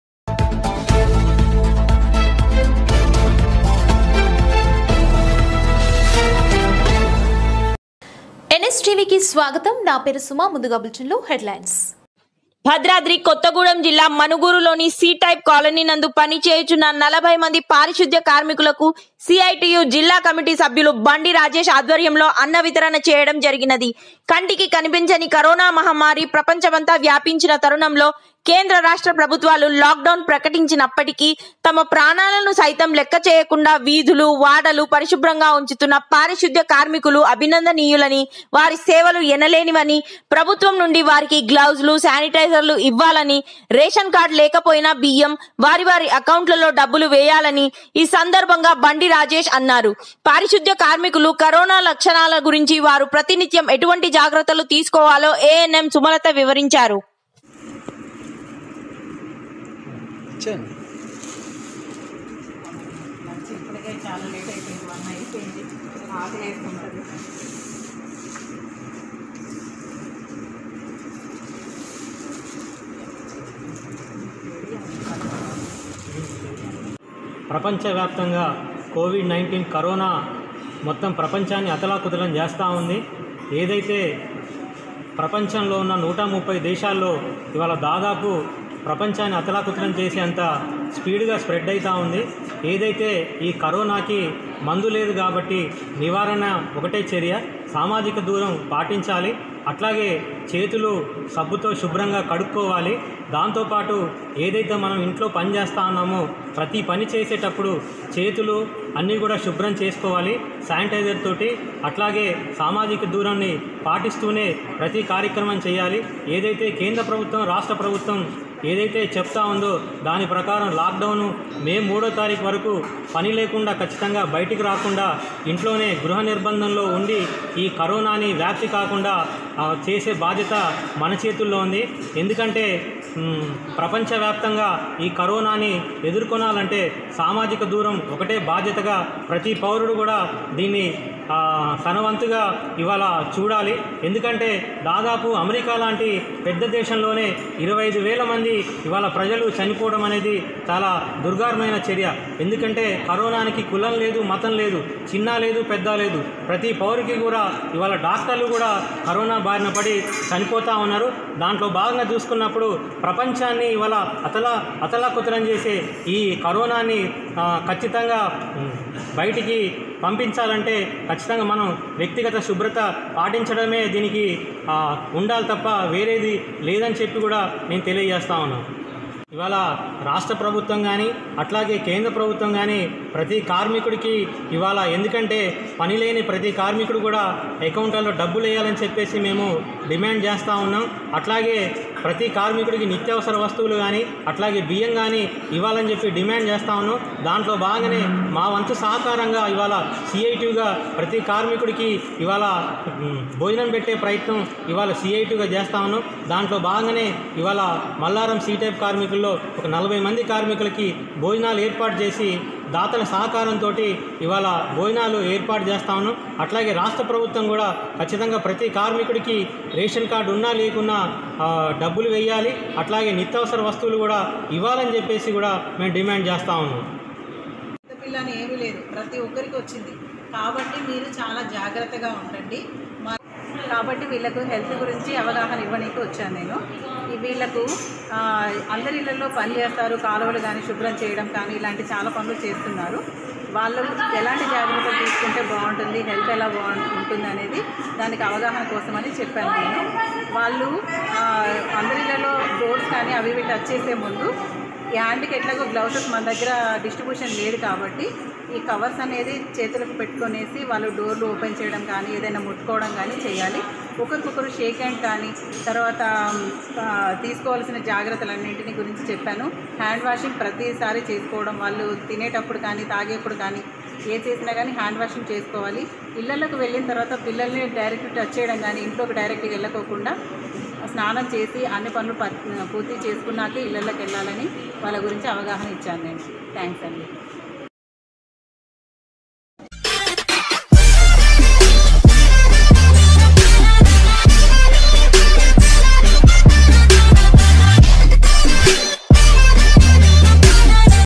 Nstv#CITU ఆధ్వర్యంలో 40 మంది కార్మికులకు అన్న వితరణ# భద్రాద్రి కొత్తగూడెం జిల్లా మణుగూరు లోని *సీ టై
Nstv#భద్రాద్రి కొత్తగూడెం జిల్లా మణుగూరు లోని *సీ టైప్ కాలనీ* నందు పనిచేయుచున్న పారిశుద్ధ్య కార్మికులకు సిఐటియు ఆధ్వర్యంలో 40 మంది కార్మికులకు సిఐటియు జిల్లా కమిటీ సభ్యులు బండి రాజేష్ ఆధ్వర్యంలో అన్న వితరణ చేయడం జరిగినది కంటికి కనిపించని కారోన  మహమ్మారి ప్రపంచమంతా వ్యాపించిన తరుణంలో కేంద్ర రాష్ట్ర ప్రభుత్వాలు లాక్ టౌన్ ప్రకటించినప్పటికీ ని తమ ప్రాణాలను సైతం లెక్క చేయకుండా వీధులు వాడలు పరిశుభ్రంగా ఉంచుతున్న పారిశుద్ధ్య కార్మికులు అభినందనీయులని వారి సేవలు ఎనలేనివి  అని  ప్రభుత్వం నుండి వారికి చేతికి గ్లౌజులు శానిటైజర్స్ ఇవ్వాలి రేషన్ కార్డ్ లేక పోయినా బియ్యం, వారివారి అకౌంట్స్ లో డబ్బులు